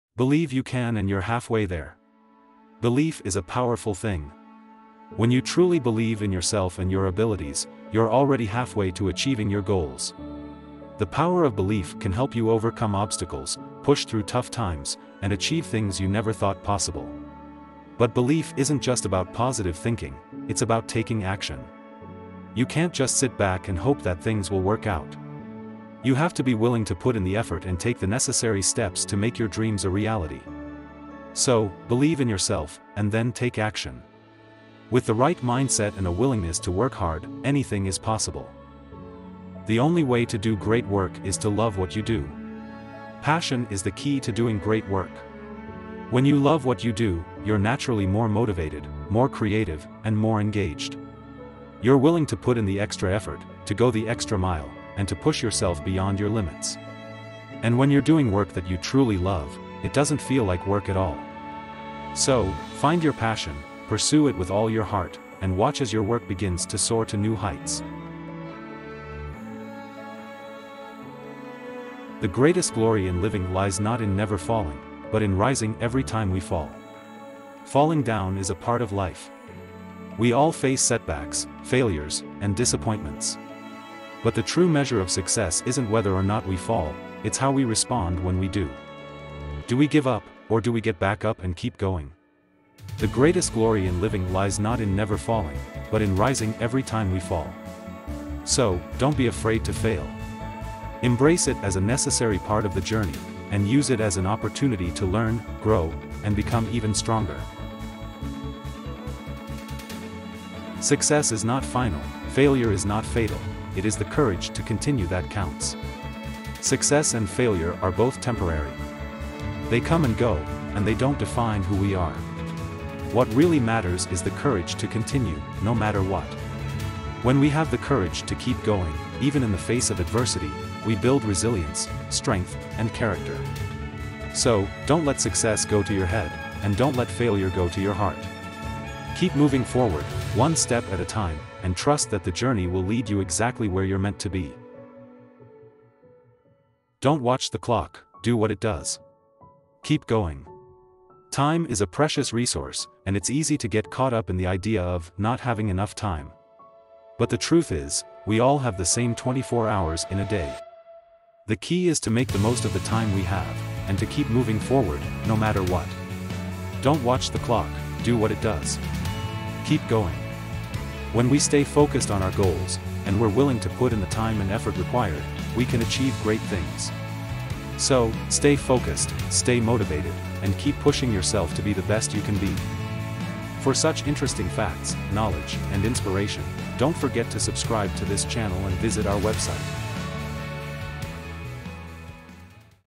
5 Inspiring Quotes to Help You Achieve Your Goals
In this video, we share 5 inspiring quotes to help you stay motivated and achieve your goals. From the power of belief to the importance of perseverance, these quotes will inspire you to push through obstacles and reach new heights. Whether you're struggling to stay focused or just need a little boost of motivation, these quotes will help you stay on track and keep moving forward.